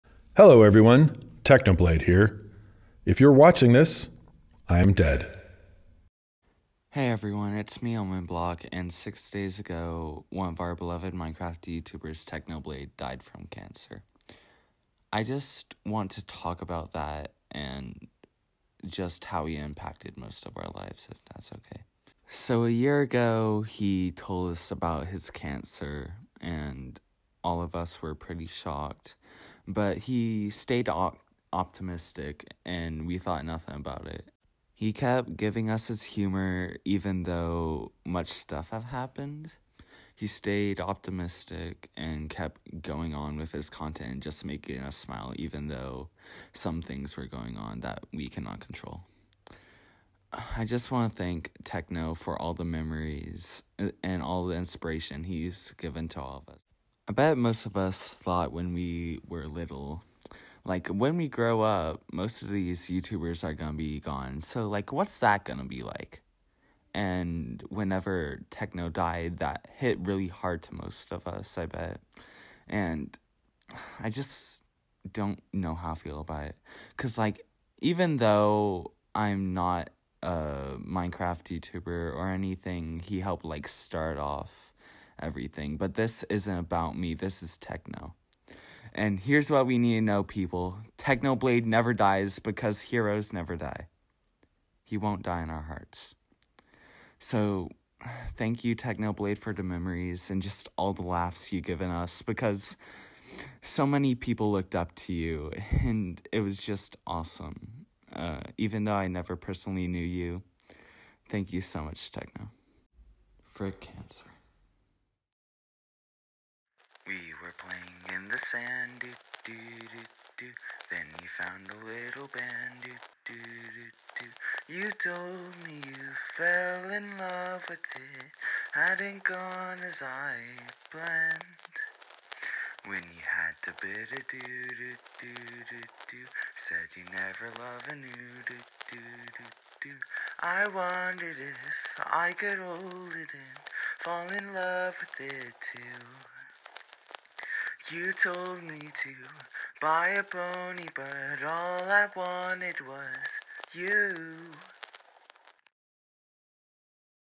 A Tribute to Technoblade
This video is a tribute to Technoblade and what’s he’s done for us, Technoblade impacted a lot of our lives and made us smile in the hard times. I didn’t know what to say at first, but I do know what to say to cancer,

Frick cancer.

Footage used at the beginning by @Technoblade (obviously lol)